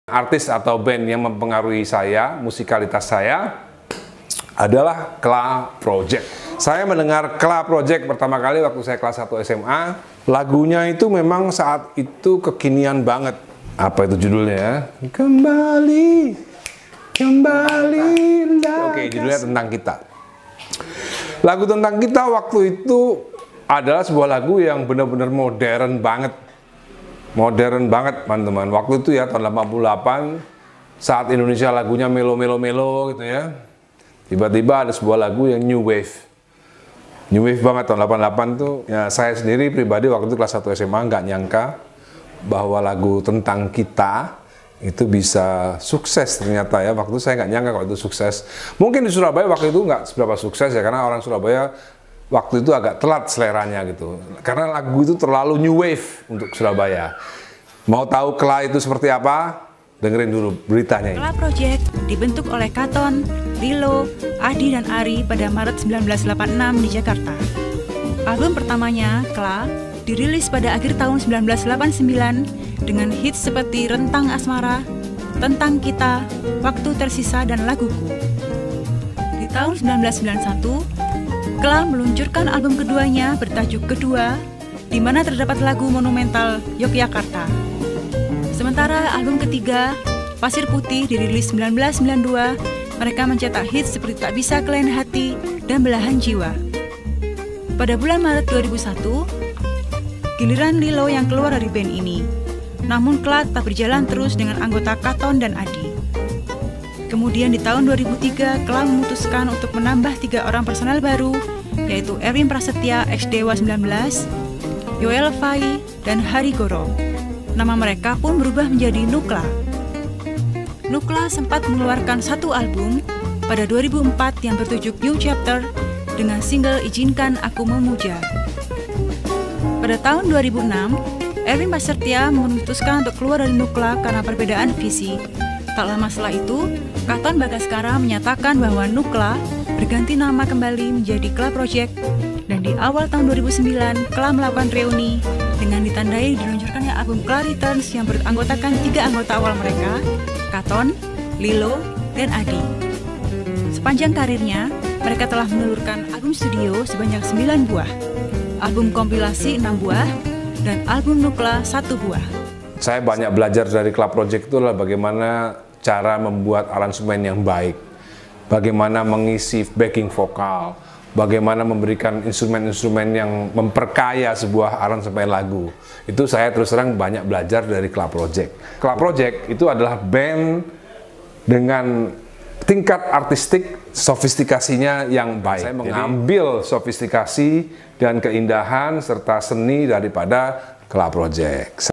KLA PROJECT di mata Ahmad Dhani ‼️
Source : Video Legend

Ini adalah potongan Video dari Video panjang di channel Video Legend musisi yang mempengaruhi Ahmad Dhani. Pada sesi kali ini saya mengambil KLA PROJECT.